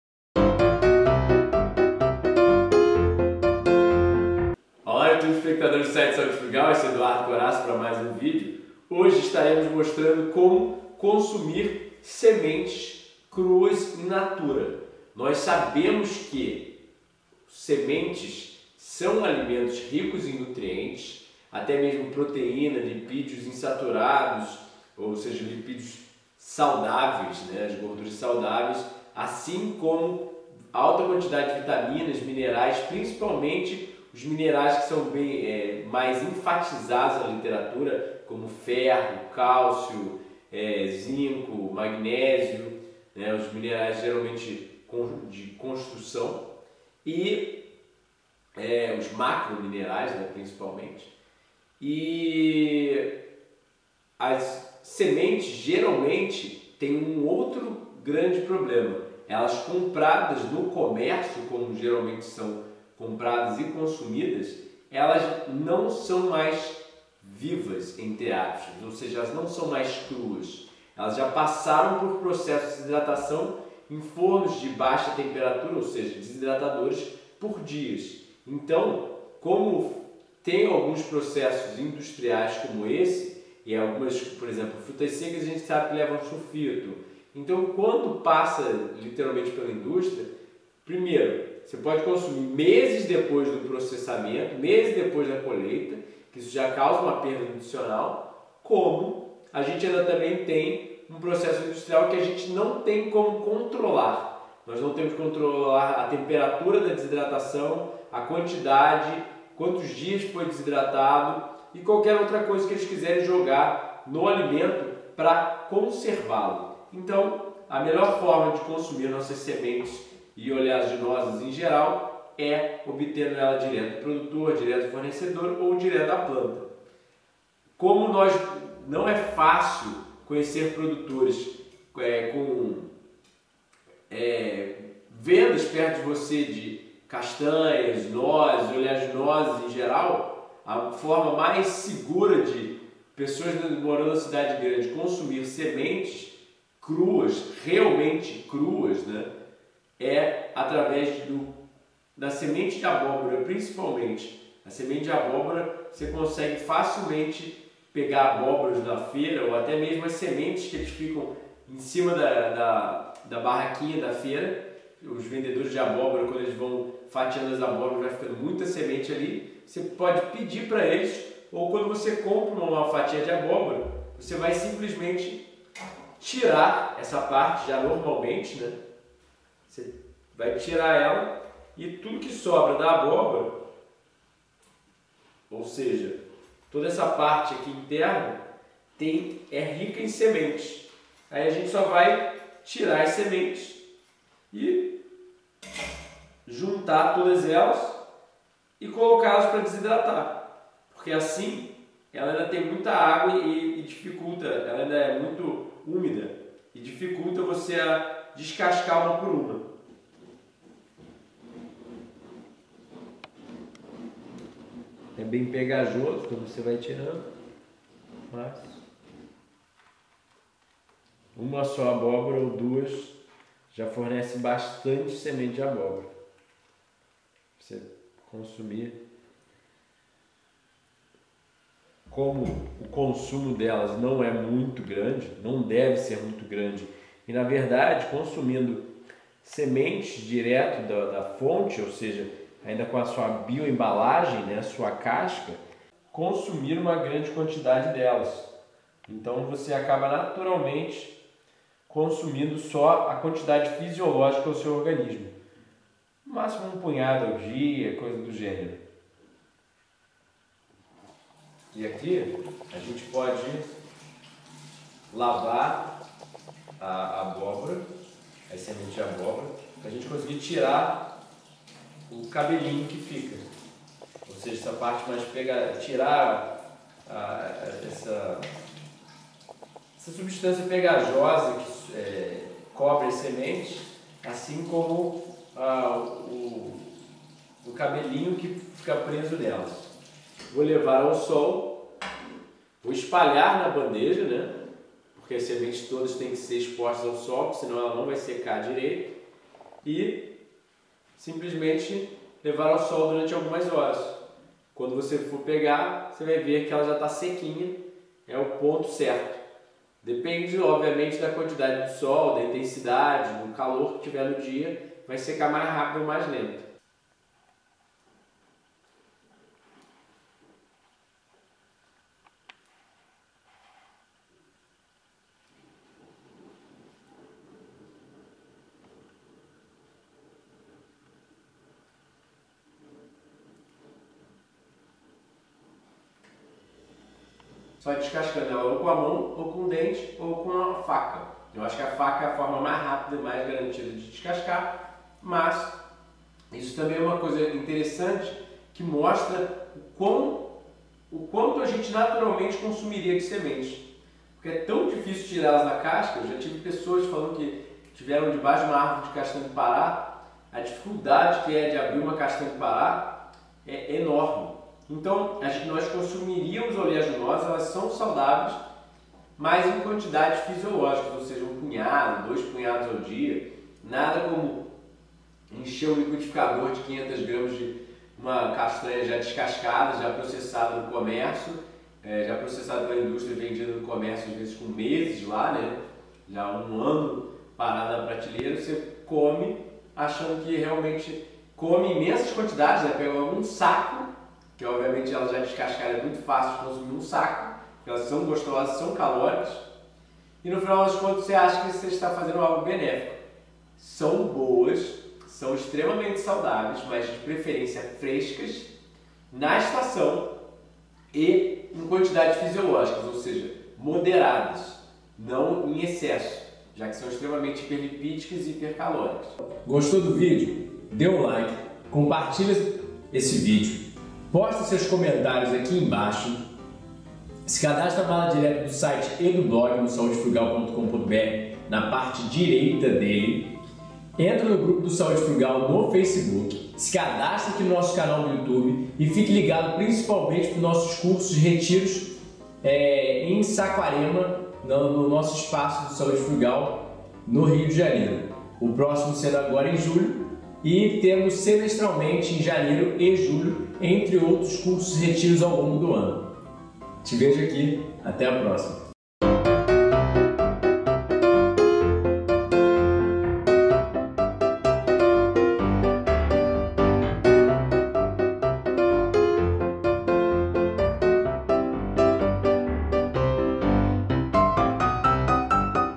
SEMENTE DE ABÓBORA A MELHOR SEMENTE FONTE DE PROTEÍNA e minerais Petisco vegano
Eduardo Corassa, do site Saúde Frugal, ensina a desidratar e consumir sementes de abóbora. Como consumir uma oleaginosa super saudável e sem passar pela usual industrialização que elas passam e a consequente perda de nutrientes.